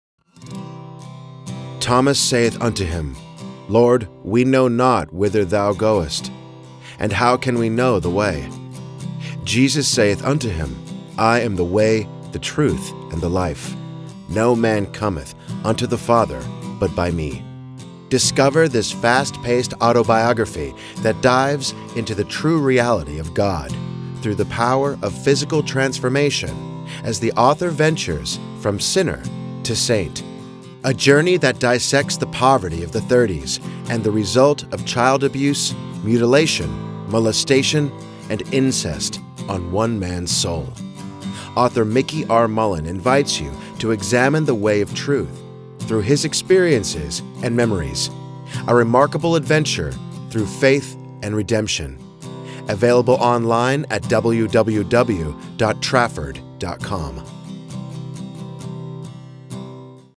The Way The Truth and The Life by Mickey R. Mullen - 60 sec
Standing there, something hit me in my back and knocked my heart out of me to the right. The suit coat went limp, with my body feeling sunken. At first, it had occurred to me, I was shot. The impact was so great my right foot went forward a half step. Finding out my neck was stiff, out of the corner of the left eye I saw and felt a long stream of white misty light entering my back and my body was filling up. According to scripture, "replacing my heart." St Matthew 9 v 16-17.